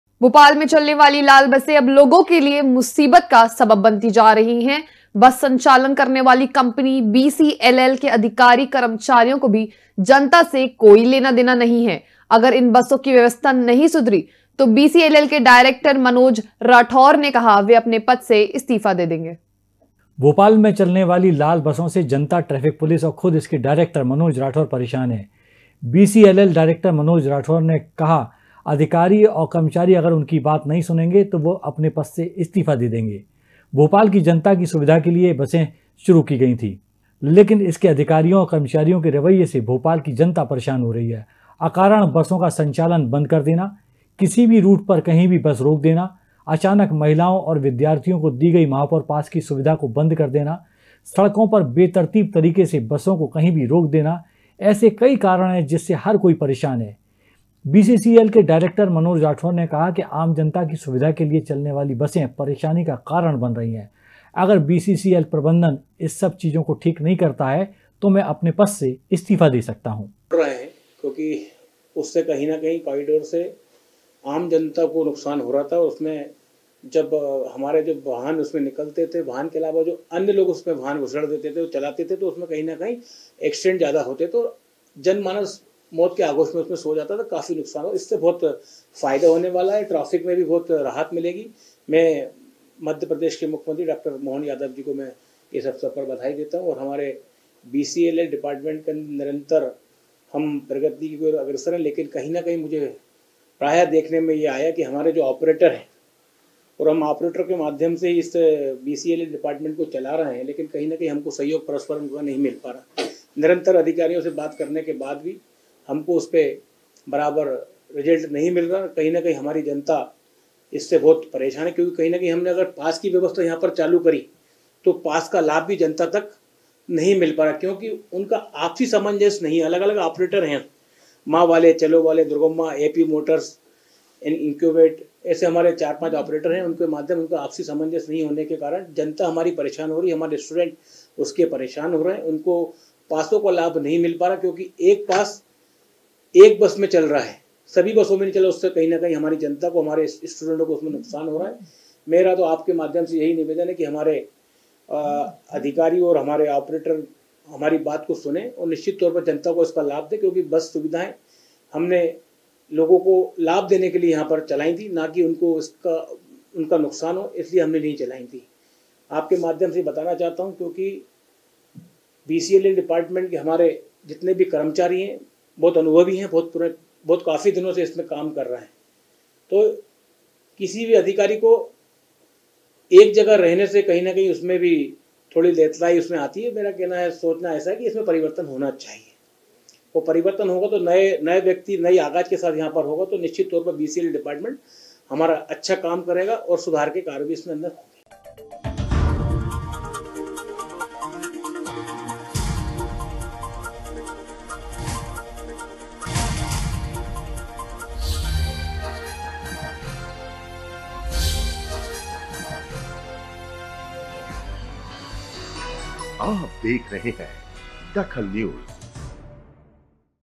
DAKHAL NEWS BCLL डायरेक्टर मनोज राठौर ने दिया बड़ा बयान
BCLL डायरेक्टर  मनोज राठौर ने दिया बड़ा बयान
जनता की सुविधा के लिए अपने पद से इस्तीफा दे सकते हैं